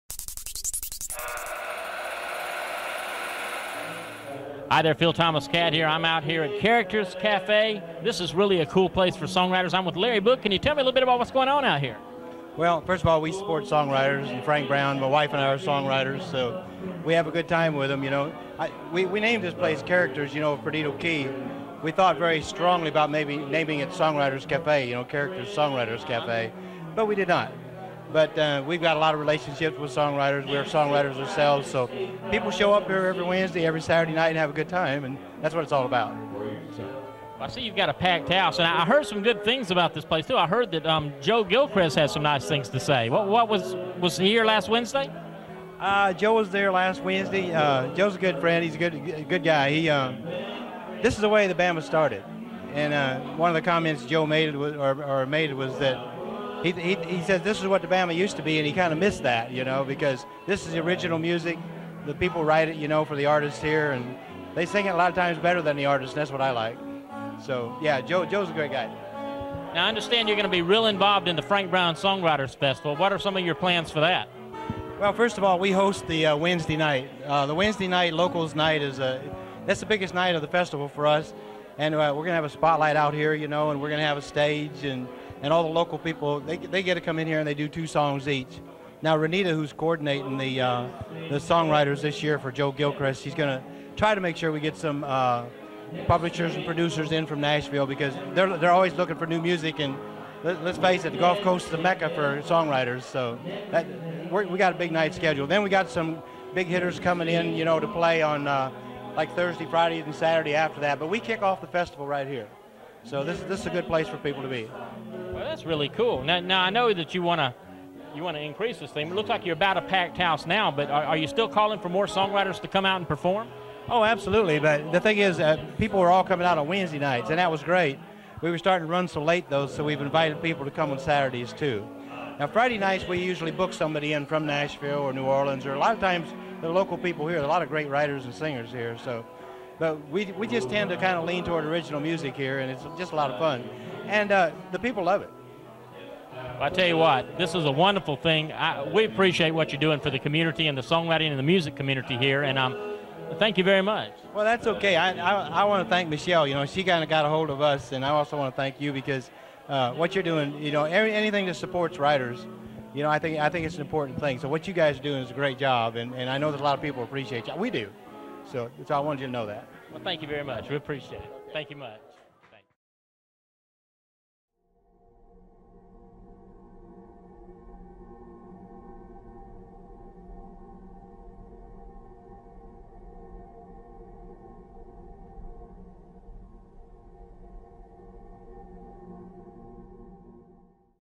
The Larry Book Interview
In 2004 Larry & Gail Book opened Characters Café, a place for songwriters. It was a very active place that Flora-Bama owner Joe Gilchrist said reminded him of the early years of the Flora-Bama. Sadly Hurricane Ivan blew the place away on September 16, 2004.